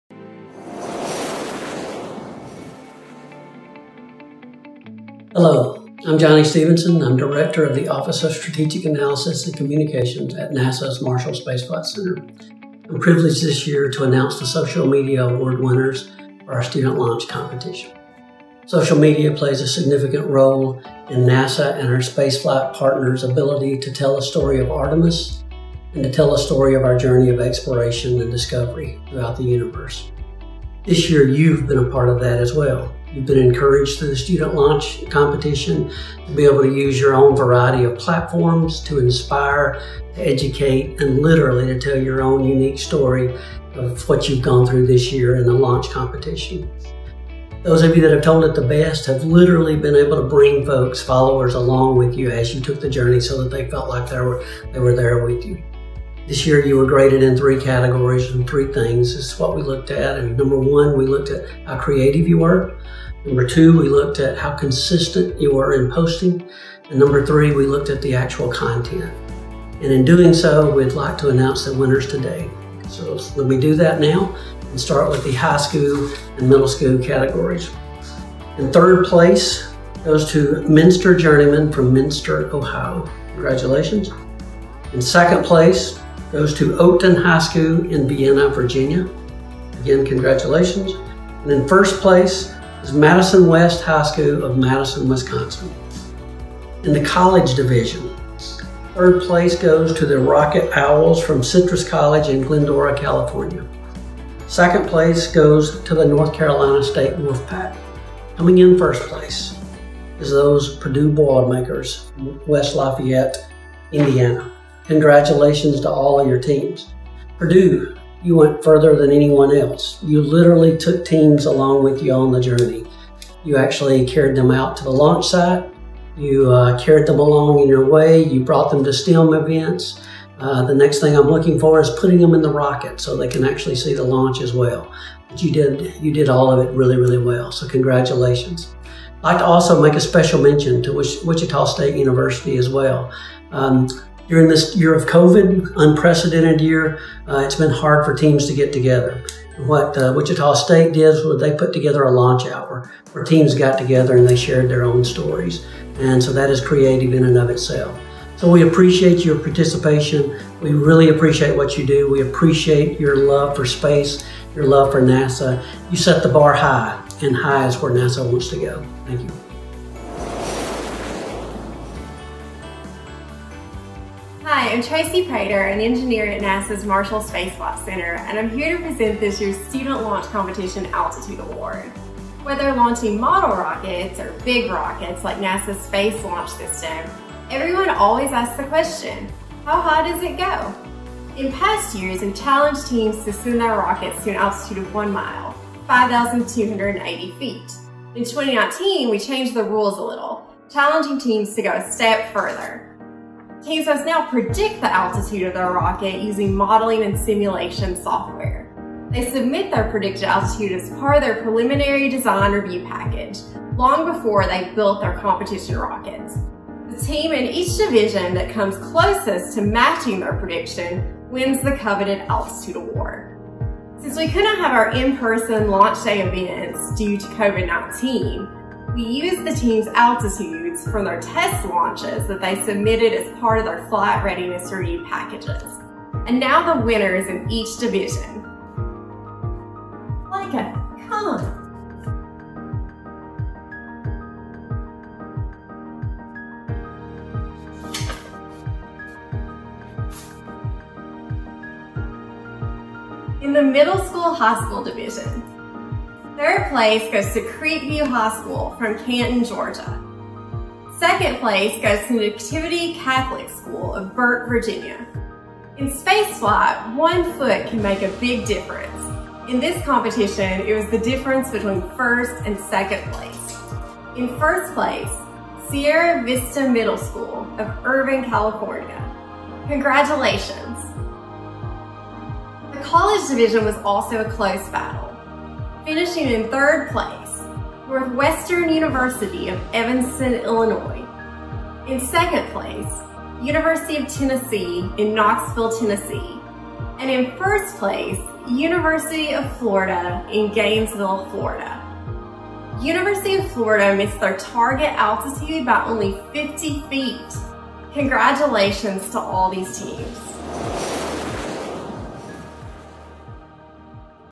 2020 Student Launch Awards Ceremony - Social Media and Altitude Awards
Our StudentLaunch teams predicted weeks in advance just how high their rockets would fly, while telling their unique stories on social media. The teams that launched closest to their predictions and best used social media won the Social Media and Altitude awards.